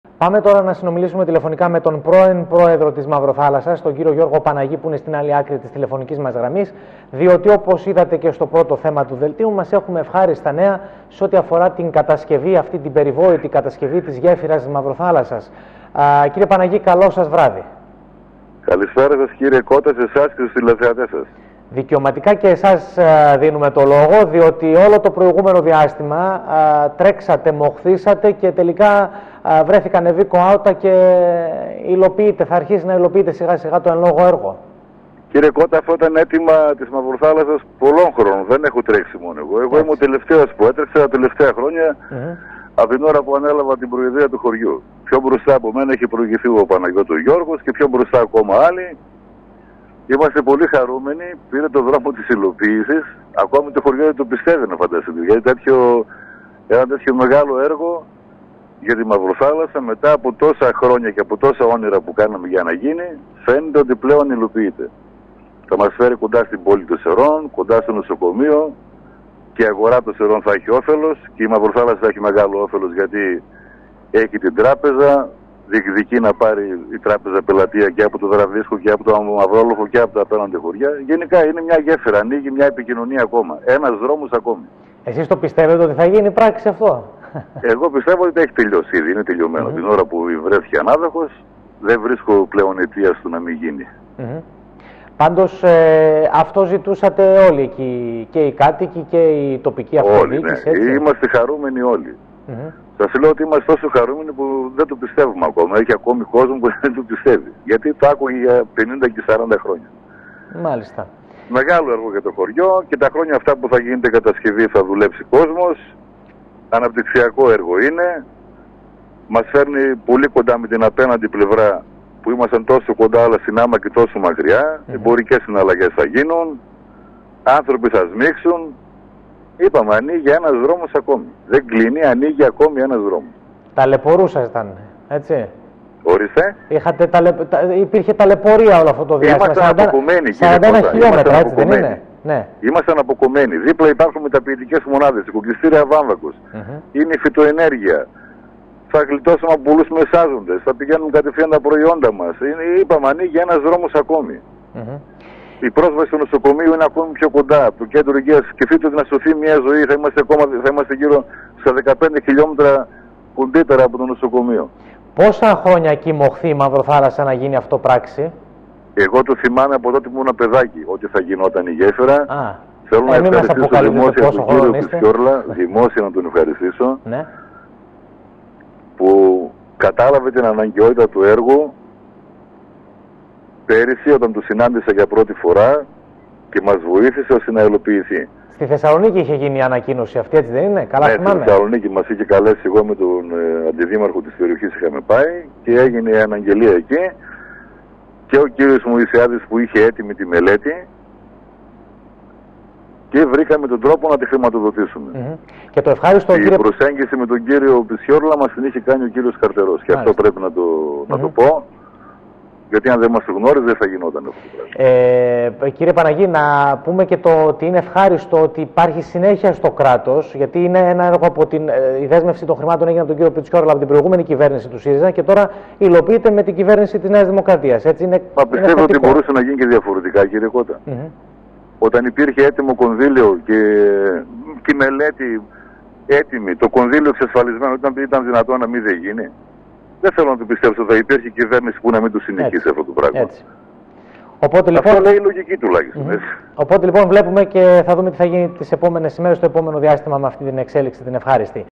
Παναγής για γέφυρα Μαυροθάλασσας Ένα πάγιο αίτημα υλοποιείται
Το έργο της Γέφυρας Μαυροθάλασσας θα ξεκινήσει σιγά σιγά να υλοποιείται αφού την περασμένη Τετάρτη 2 Σεπτεμβρίου αναδείχθηκε η ανάδοχος εταιρεία που κατασκευάσει το εν λόγω έργο προϋπολογισμού μελέτης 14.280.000 ευρώ